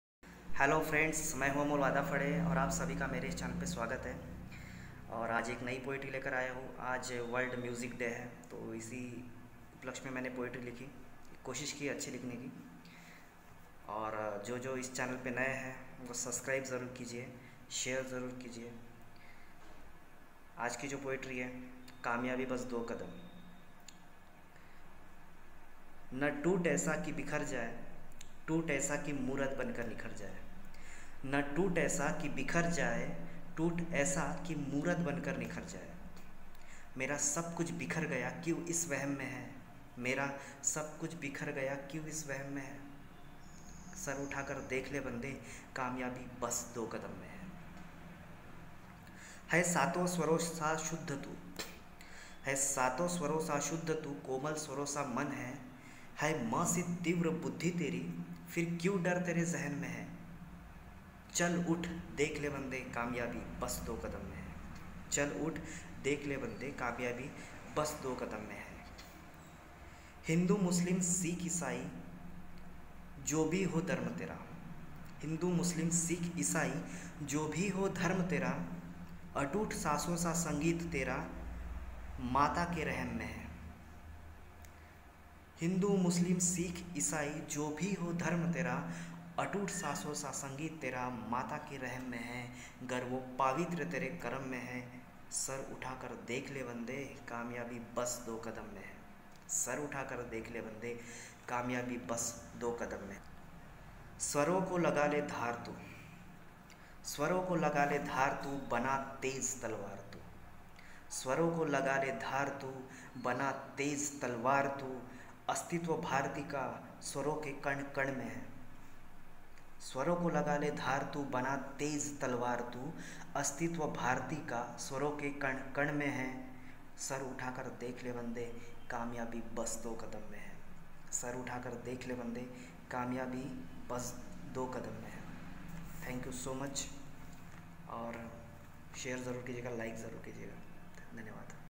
A Wonderful Poem on "World Music Day" by Amol Wadafale.
Hey! Guys again we are back with a beautiful poem on World Music day. Sorry for the late uploading. Hope you all will like it. Please give your love and support for this video as usual your give to every videos.